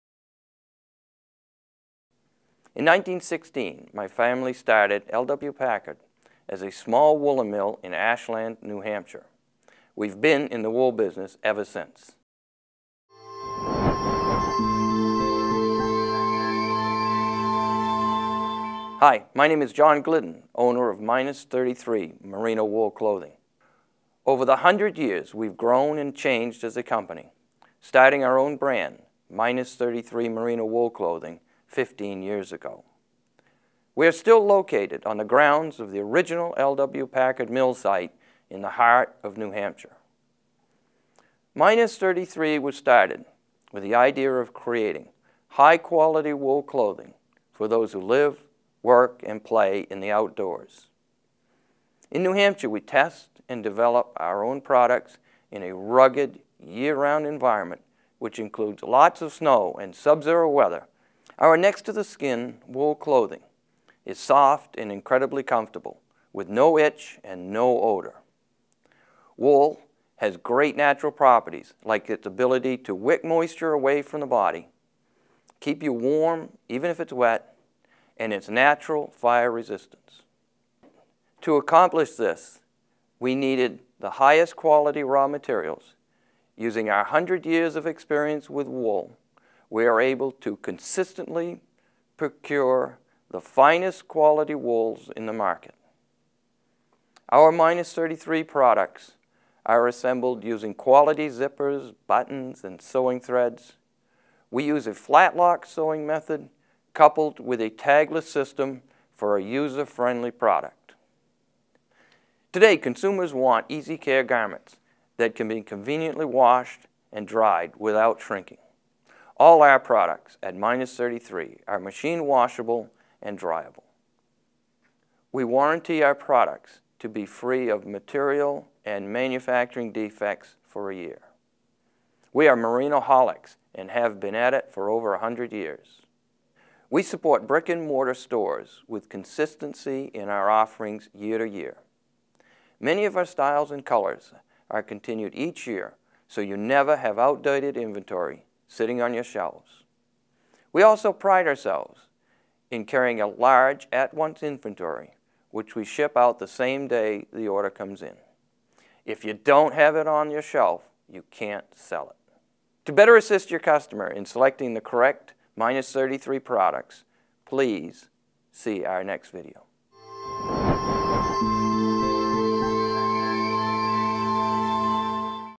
Why Minus33? [Retailer Education Series 1 of 6]
John tells about the history of Minus33, why we love wool, and how we draw from our textile history to create high quality merino layers.